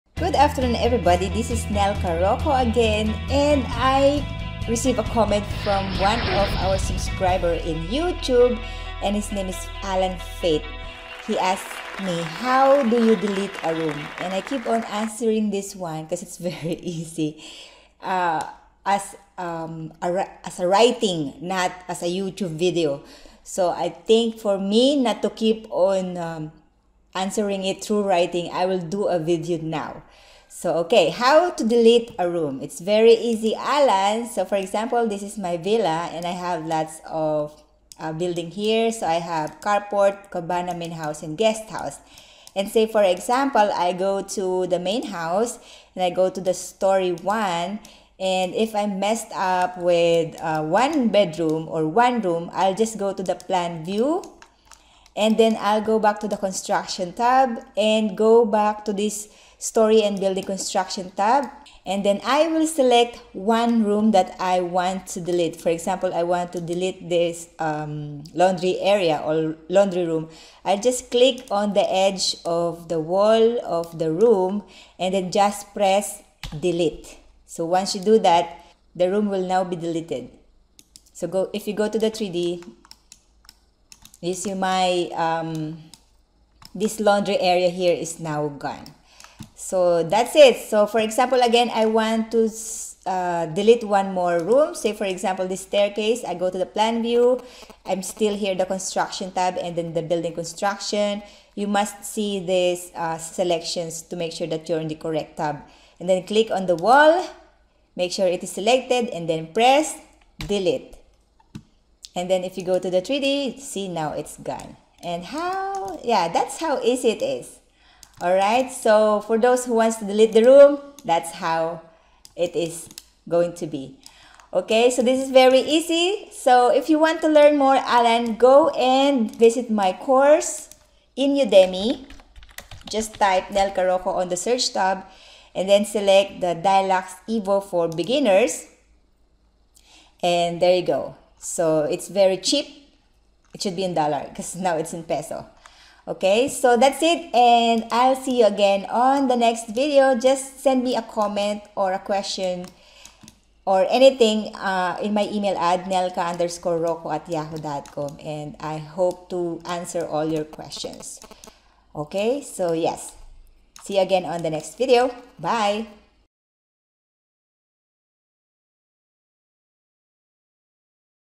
How to delete a room?
In this video, you will learn how to easily delete a room using Dialux evo software.

Enjoy learning!